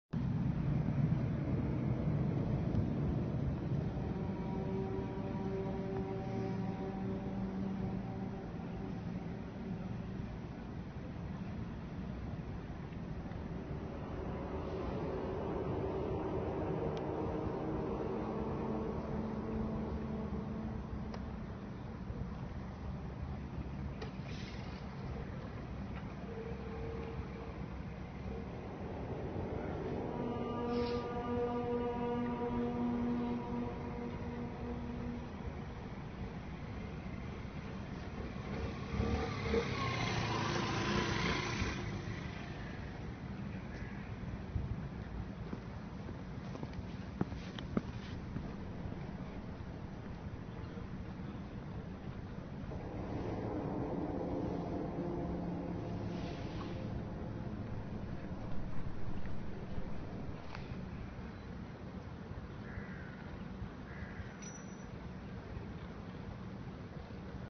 Vreemde geluiden in de lucht in Nederland, Den Haag 19 januari 2016
Wat is dit voor geluid? Opnieuw een opname van bijzonder geluid dat uit de lucht lijkt te komen, ditmaal in Den Haag, op 19 januari 2016 omstreeks 13:30. Zoals we kunnen horen op andere videos van dit fenomeen heeft het bizarre geluid iets weg van een trompet. Wat zou het kunnen zijn? Opwarming van de aarde? Buitenaards leven, een zonnevlam? In elk geval, erg angstaanjagend... Hopelijk hebben we hier snel meer duidelijkheid over.